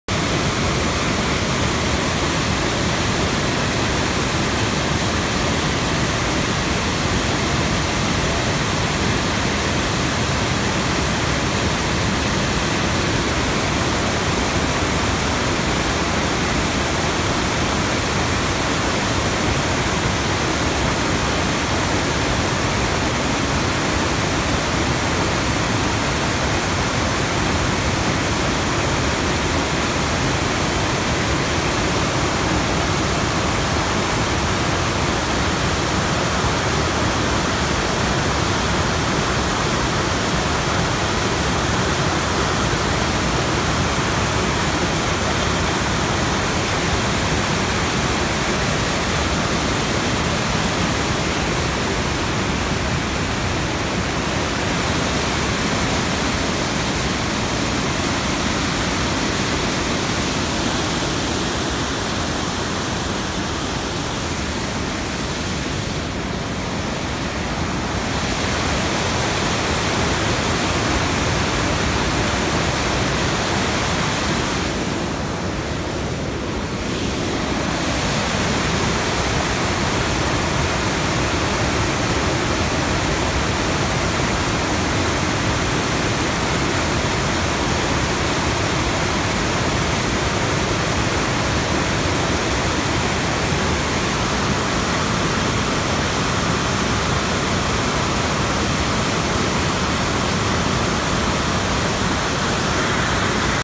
Centro Ittico
Opera di pulizia prese d'acqua centro Ittiogenico Faedo